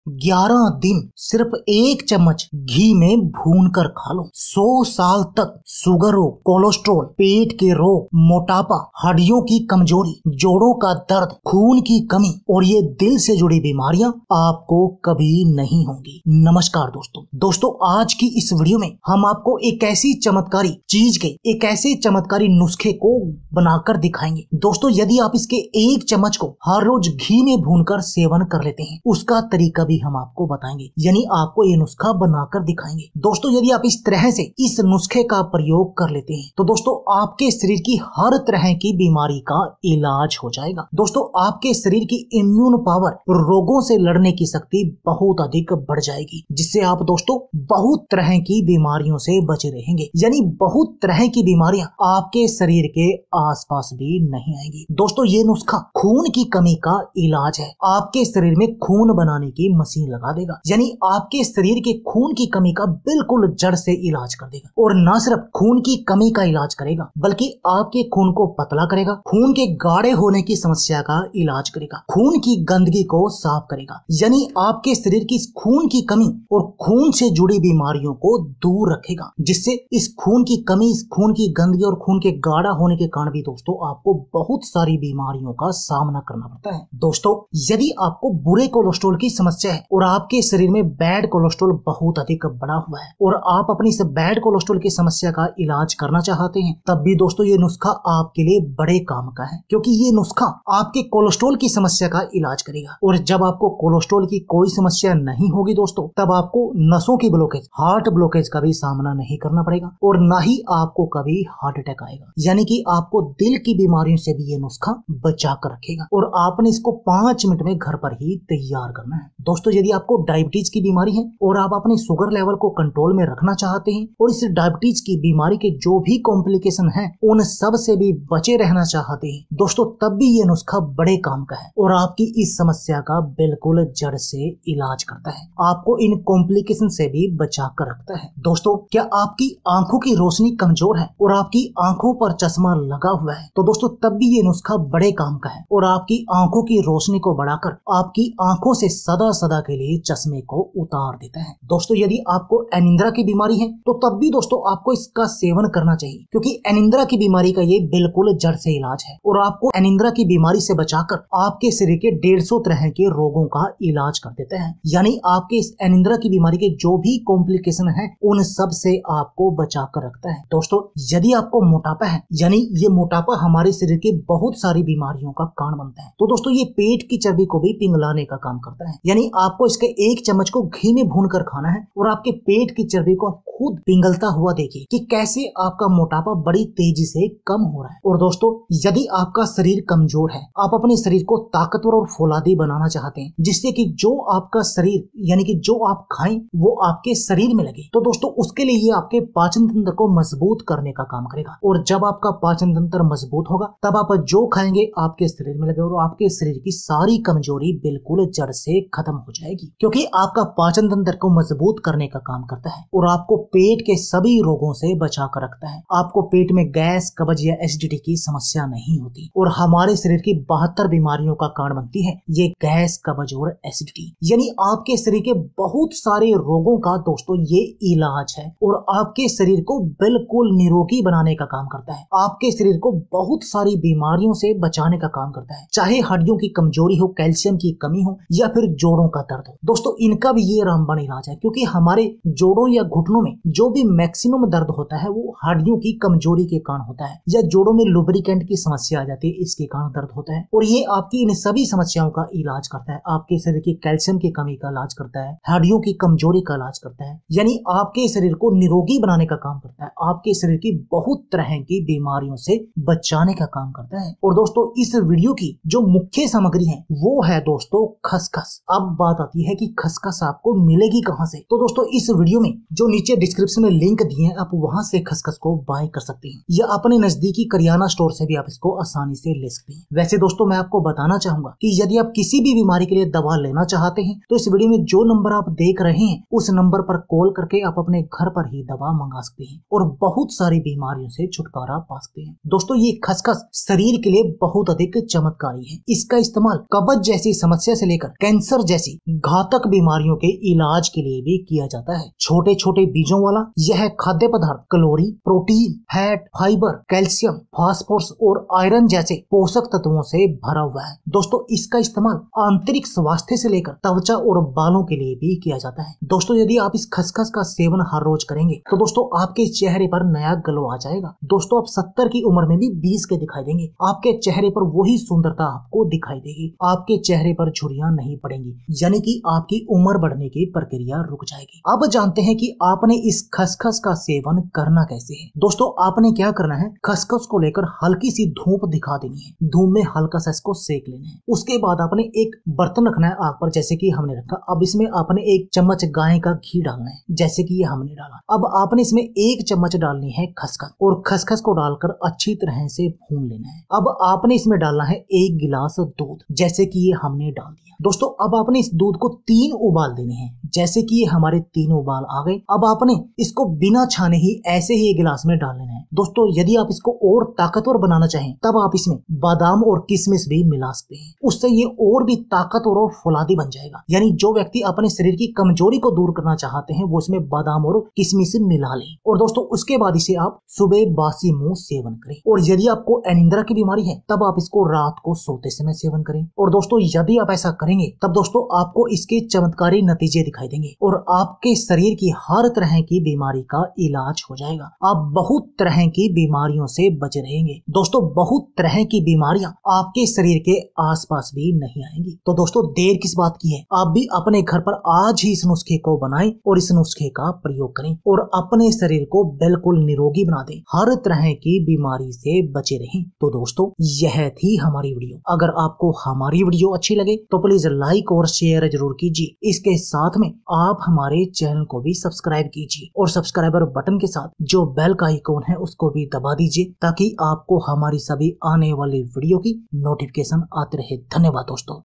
11 दिन खालों, 100 साल तक शुगर कोलेस्ट्रॉल पेट रोग मोटापा कमजोर आंखें और दिल की बीमारियाँ नहीं होंगी
11 दिन सिर्फ 1 चम्मच घी में भूनकर खालों, 100 साल तक शुगर रोग, कोलेस्ट्रॉल, पेट के रोग, मोटापा, हड्डियों की कमजोरी, जोड़ो का दर्द, खून की कमी, कमजोर आंखें, नसों की ब्लॉकेज और दिल से जुड़ी बीमारियाँ कभी नहीं होंगी